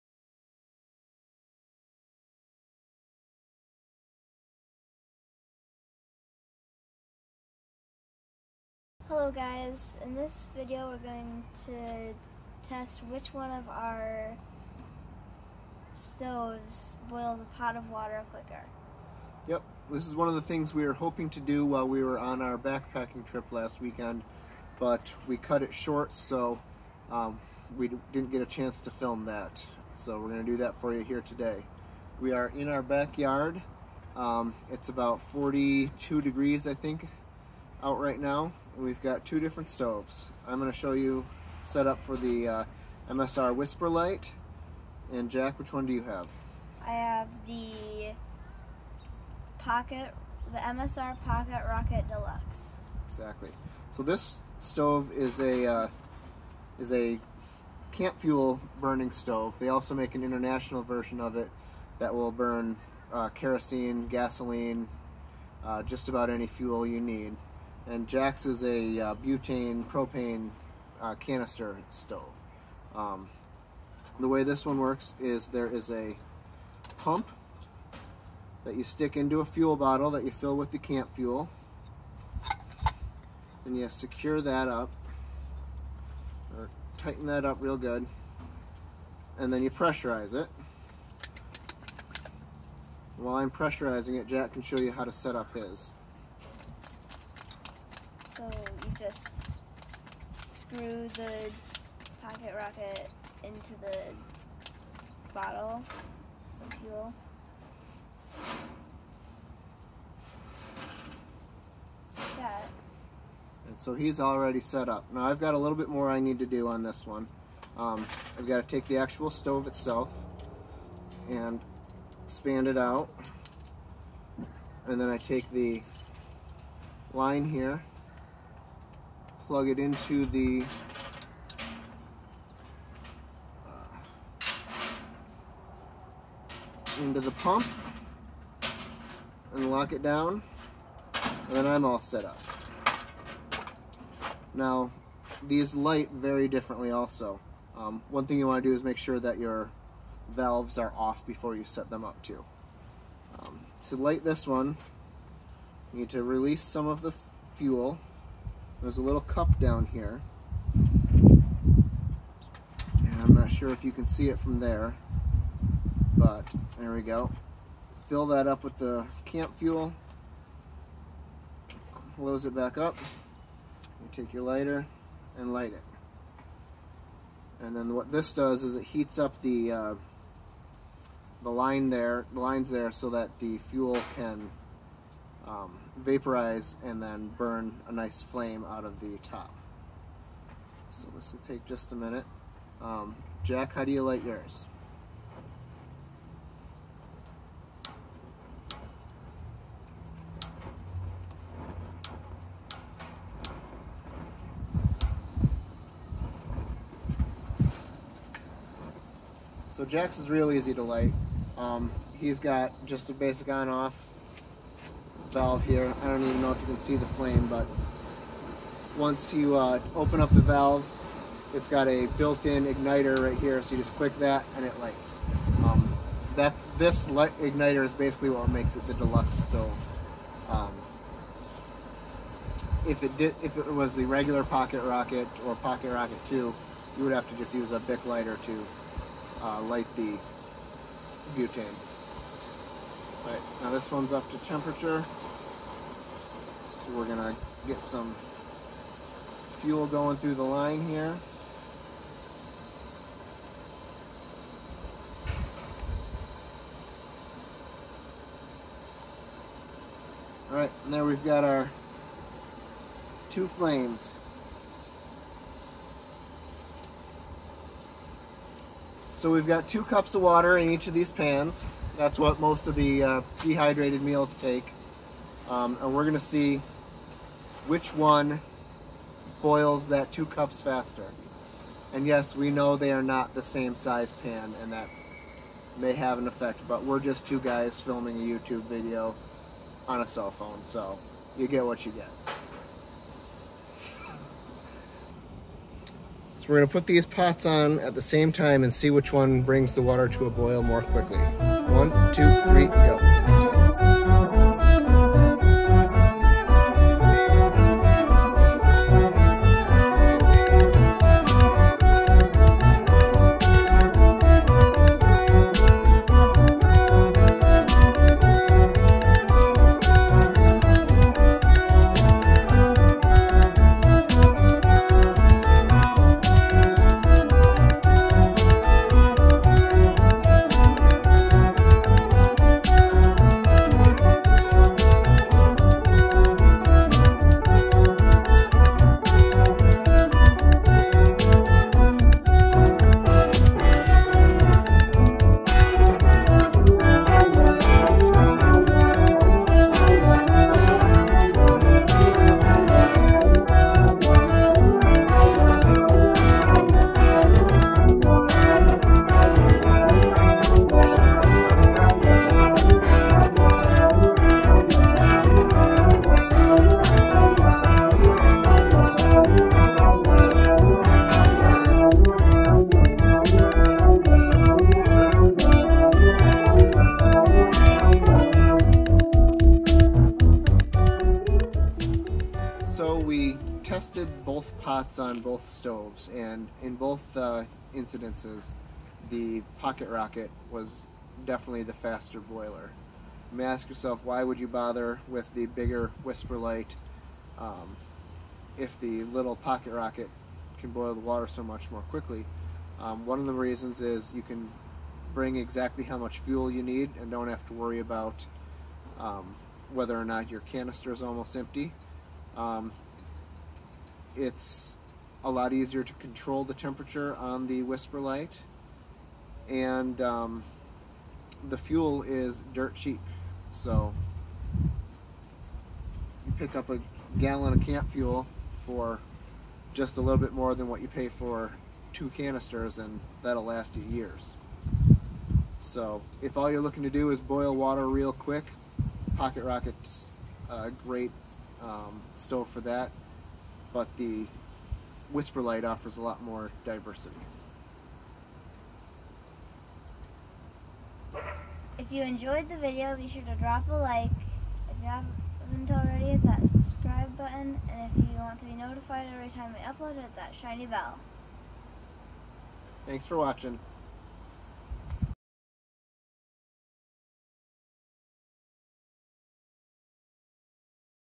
WhisperLite vs PocketRocket Deluxe
Side by side boil-off. Which will win?